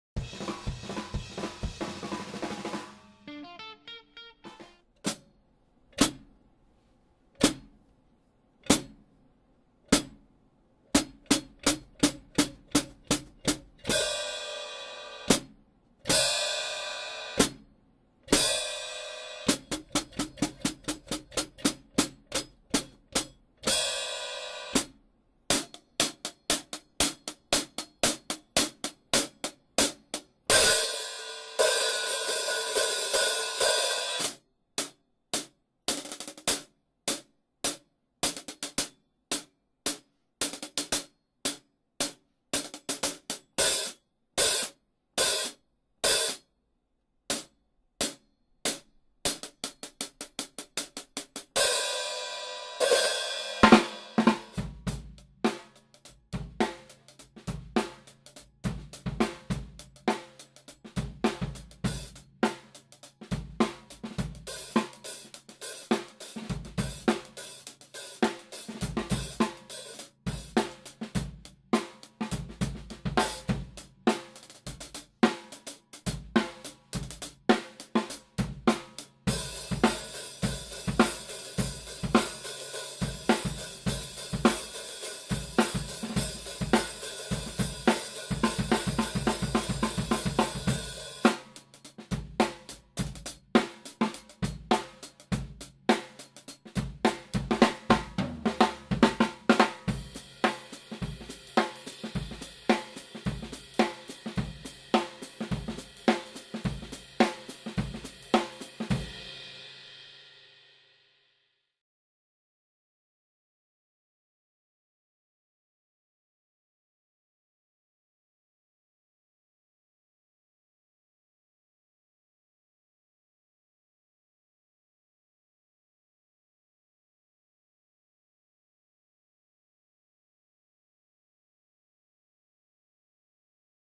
Chimbal Sabian Paragon 13" Traditional - Neil Peart - BATERACLUBE.com.br
Chimbal Sabian Paragon 13" Traditional

Partcipação especial de XUXA da banda Lestics e da Pio Parts Acessórios e Serviços.

Visite - A Primeira drumshop online do Brasil.